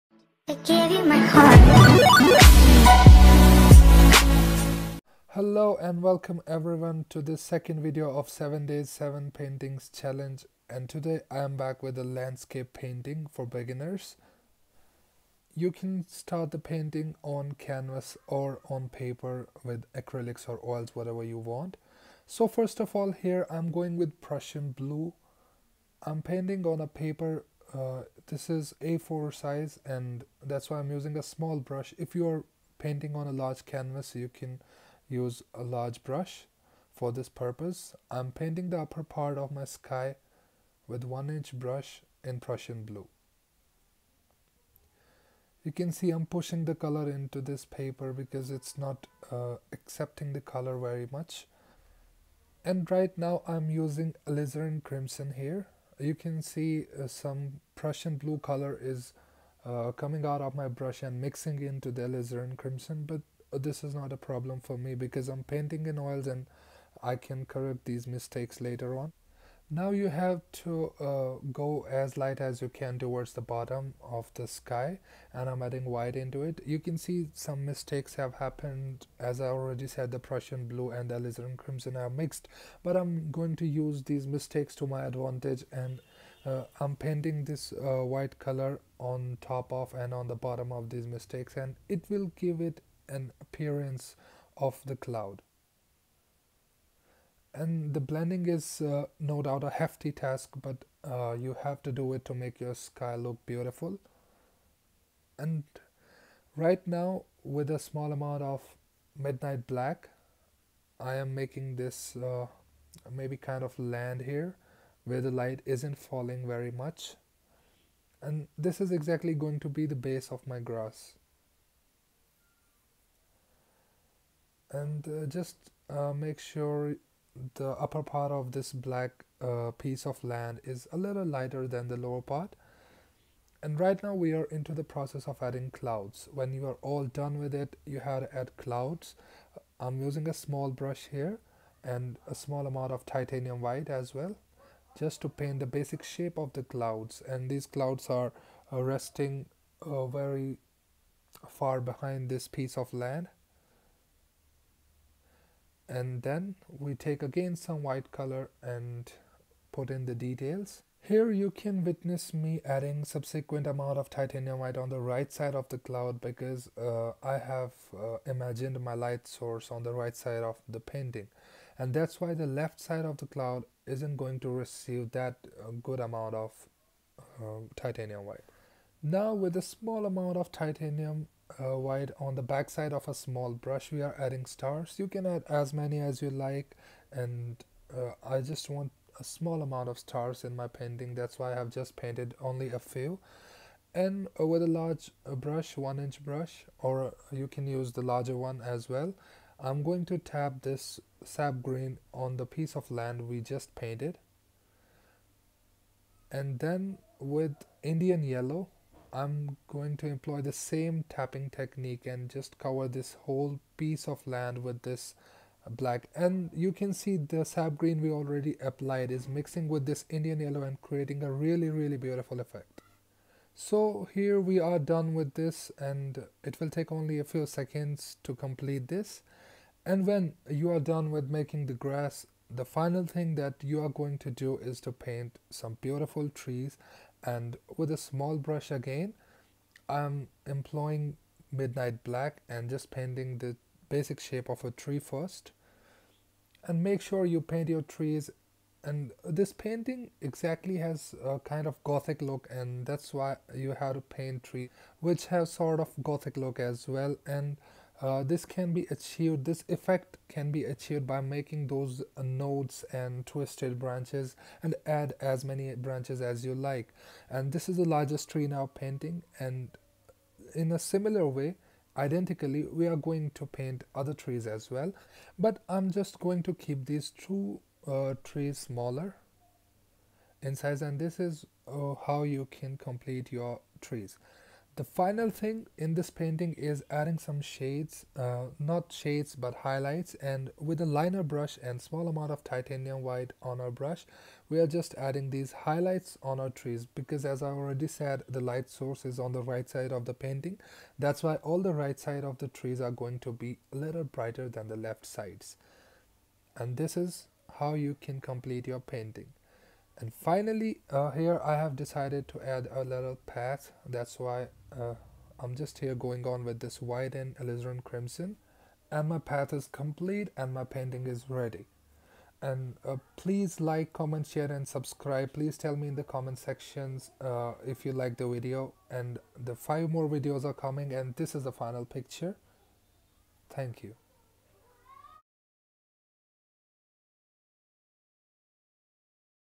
7 Days 7 Paintings Challenge For Beginners (Complete Lesson 02)
Welcome everyone and today I am here with the second video of my 07 Days 07 Paintings Challenge cum tutorial for beginners out there. Keep supporting.